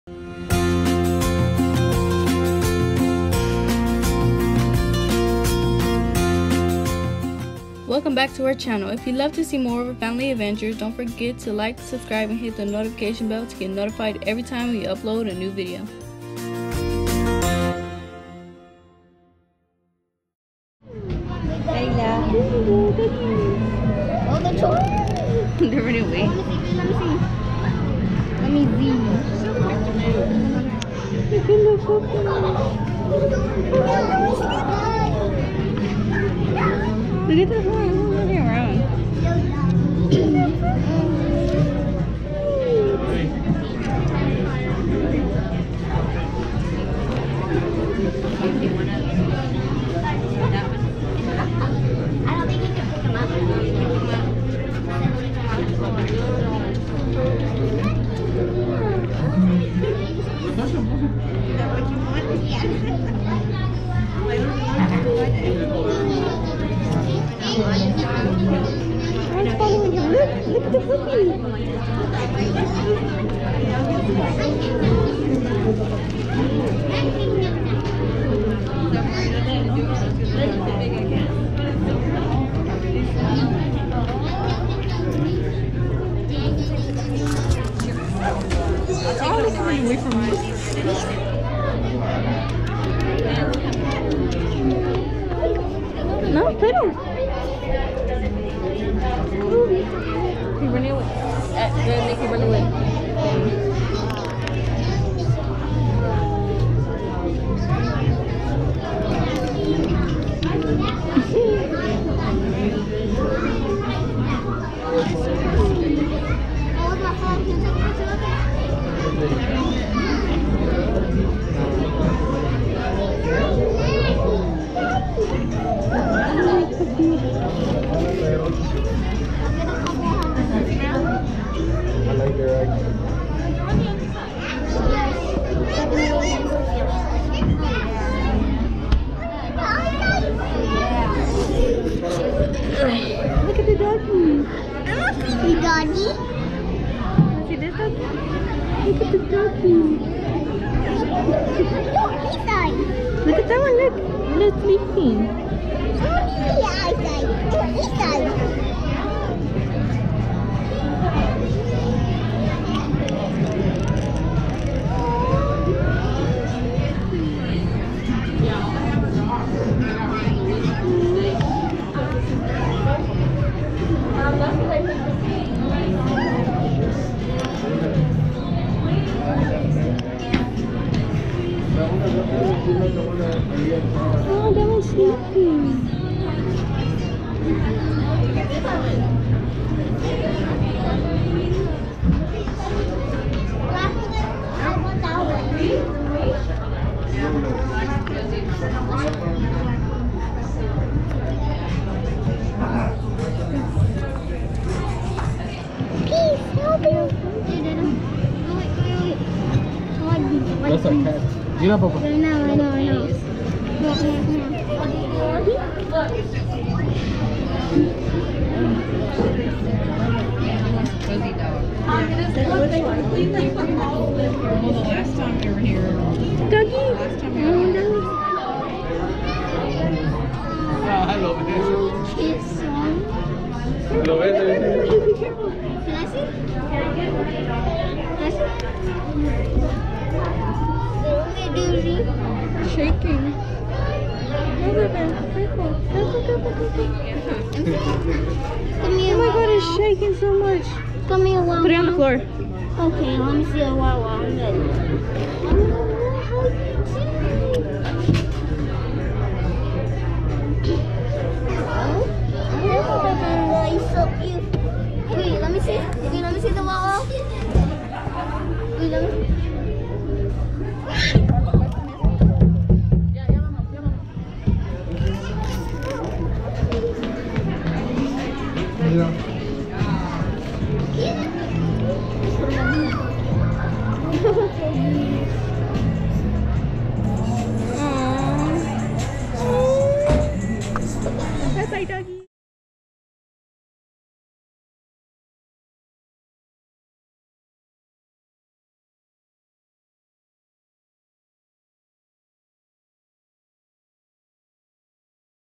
YORKIE | YORKSHIRE TERRIER | WERE WE ABLE TO CONVINCE OUR PARENTS TO BUY US A YORKIE?
On today’s vlog we went to the Pet Fair store inside the Woodlands Mall to try and convince our parents to buy us a Yorkie... were we SUCCESSFUL?